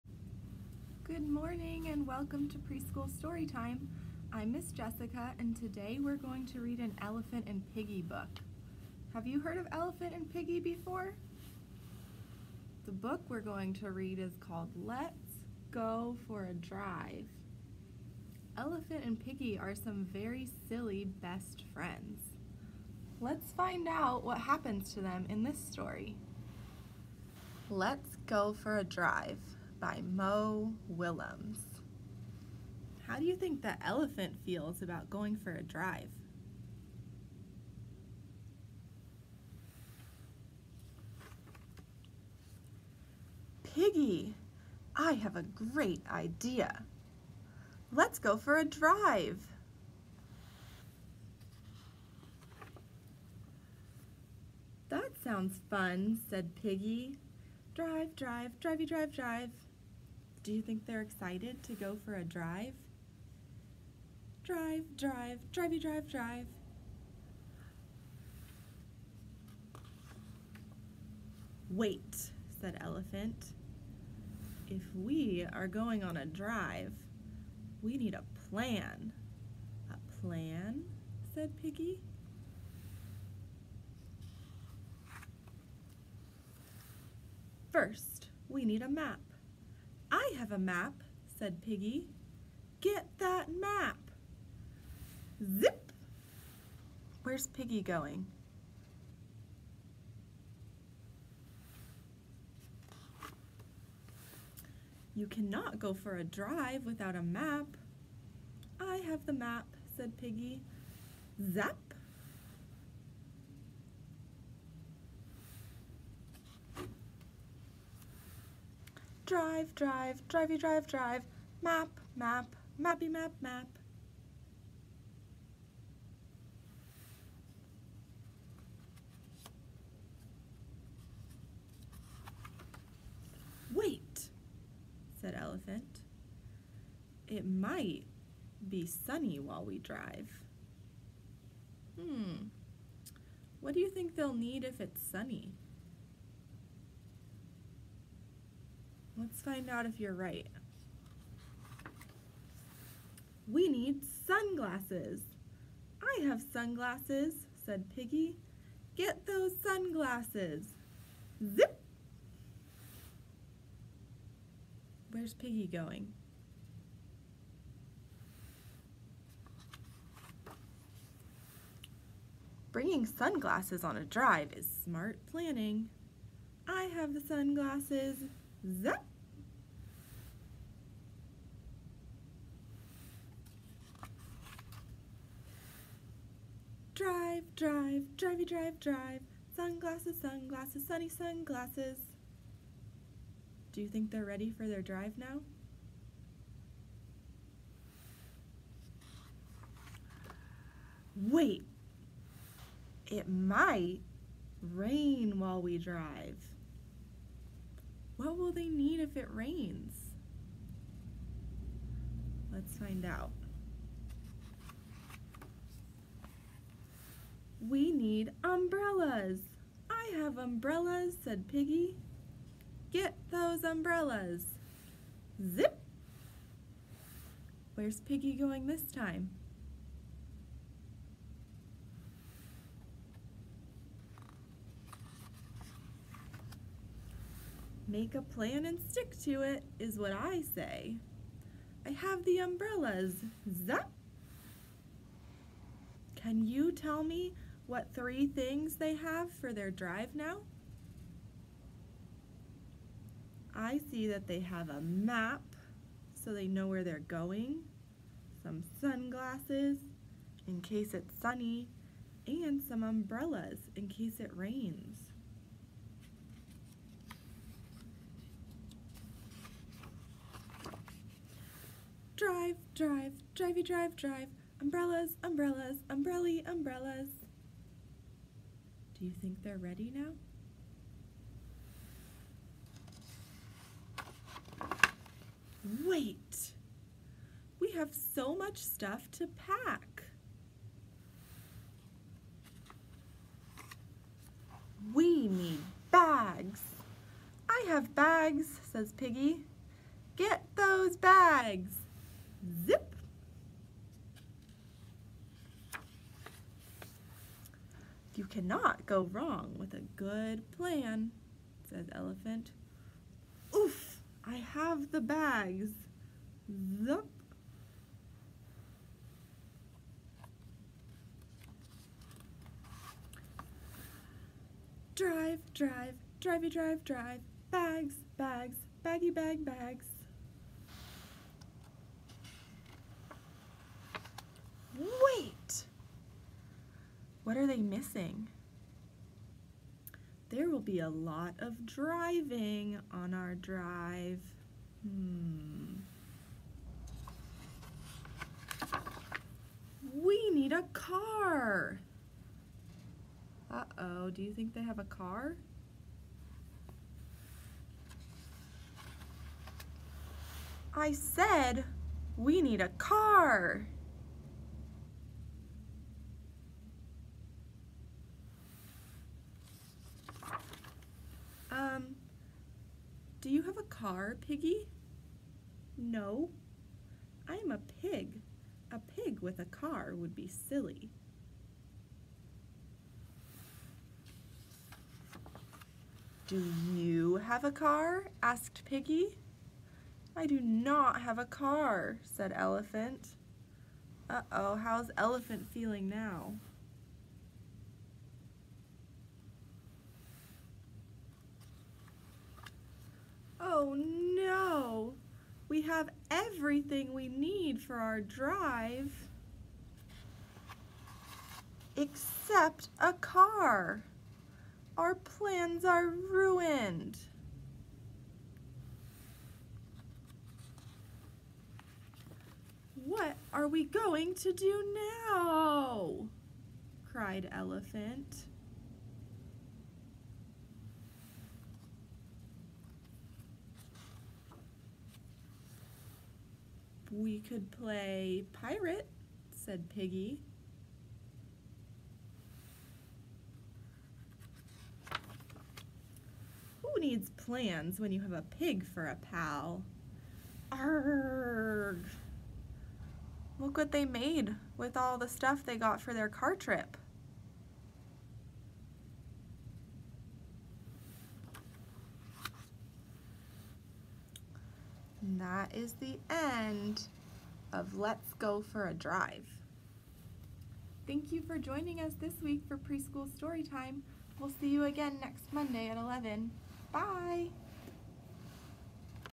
Preschool Story Time: "Let's Go for a Drive" by Mo Willems
Welcome to Preschool Story Time! This week we will be reading "Let's Go for a Drive" by Mo Willems. Join Miss Jessica to find out what Elephant and Piggie need to do to get ready for a drive!